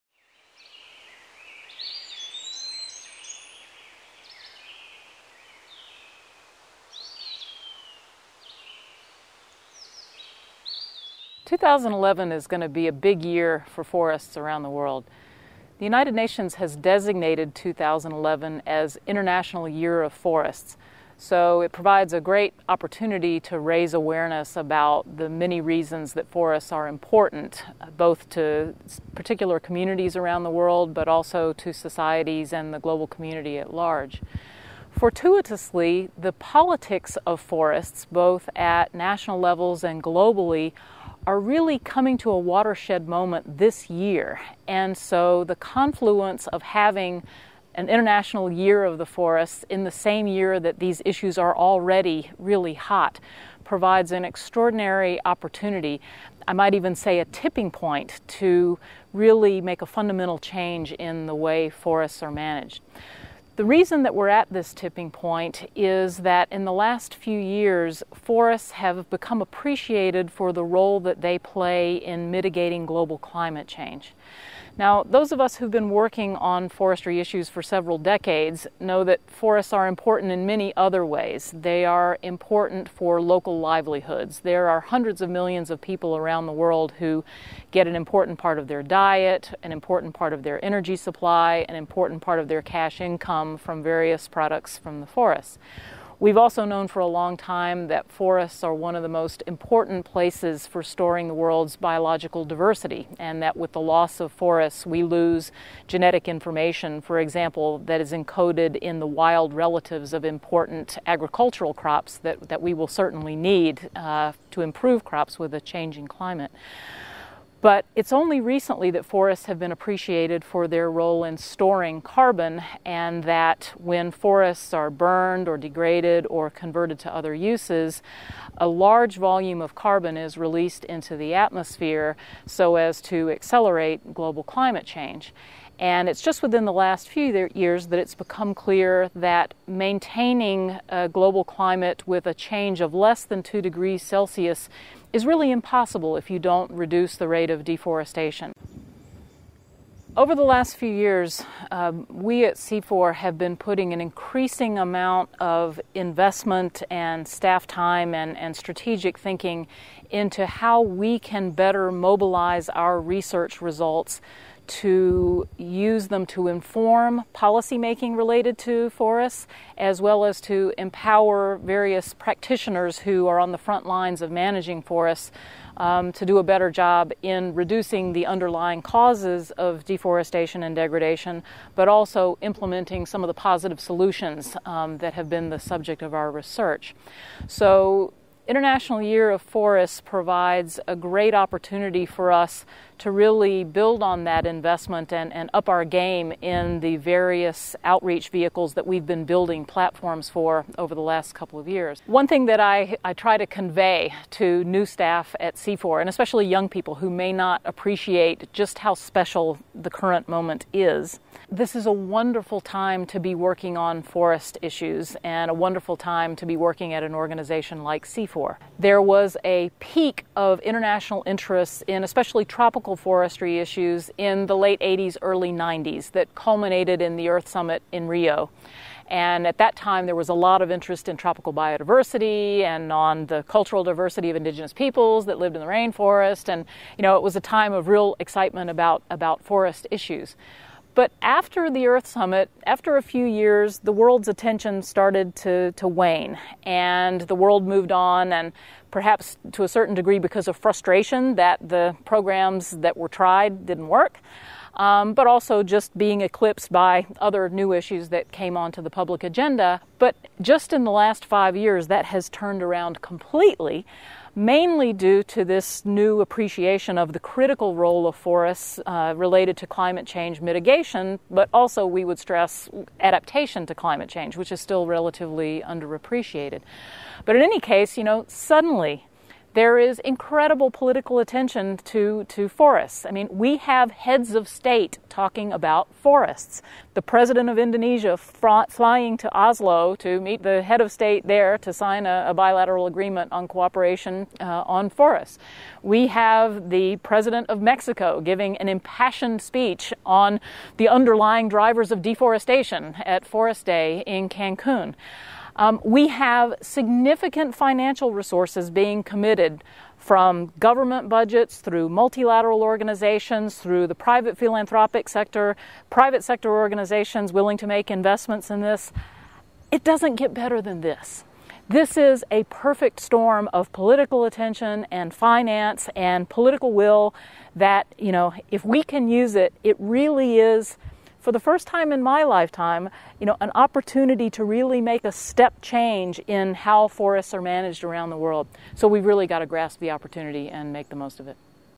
Frances Seymour - International Year of Forests
Director General of the Center International Forestry Research (CIFOR), Frances Seymour gives her views on the International Year of Forests in the lead up to the Years launch at the United Nations on 2nd February 2011 in New York. She discusses the importance of the year in the broader context of forestry issues and how CIFOR is positioned to continue its research particularly on the role forests have on climate change.